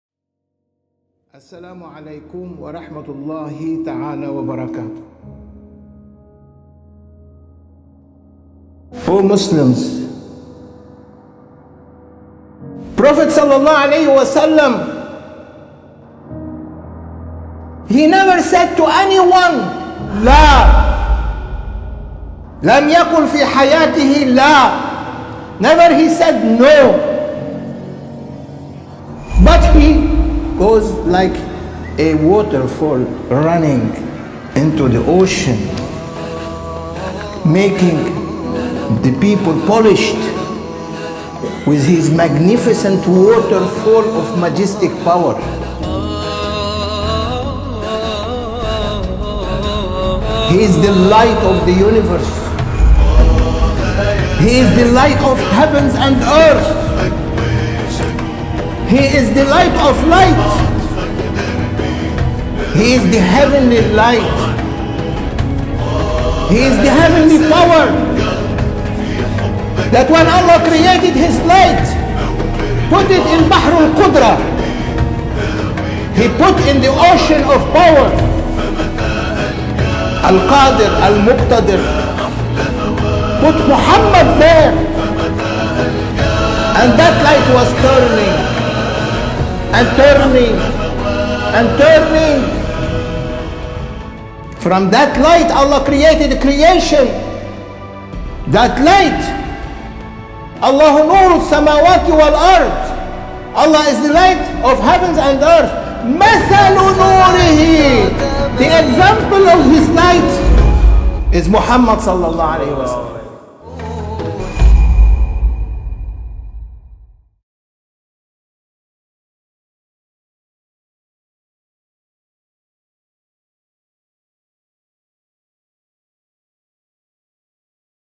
Mawlana Shaykh Hisham UK Visit Trailer | September 2014
Trailer for Mawlana Shaykh Hisham's upcoming visit to the UK (London, Birmingham and Manchester) 6th - 23rd September 2014.